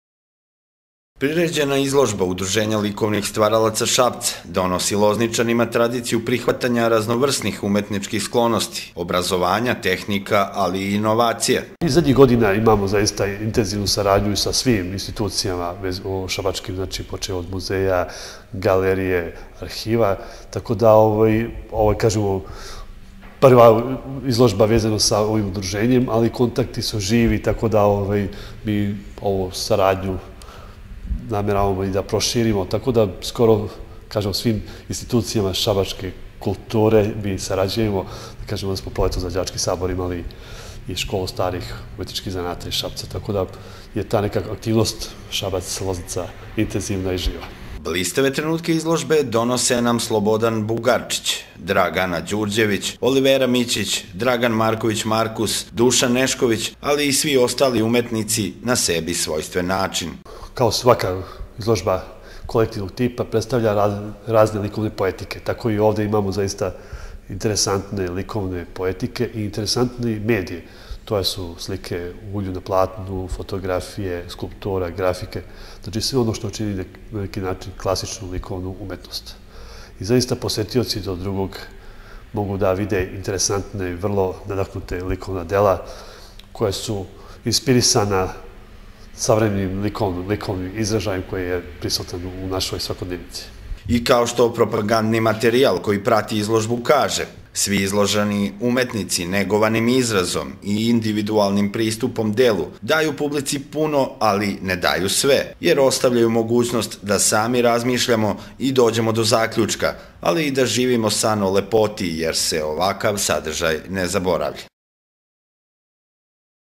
LOZNICA - IZLOŽBA "SUSRET - 12".wmv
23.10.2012. Od 18. oktobra do 2. novembra, lozničani u muzeju Jadra imaju priliku da vide izložbu Udruženja likovnih stvaralaca Šapca. Lozničanima se na ovaj način predstavlja 20 umetnika sa različitim percepcijama stvarnosti...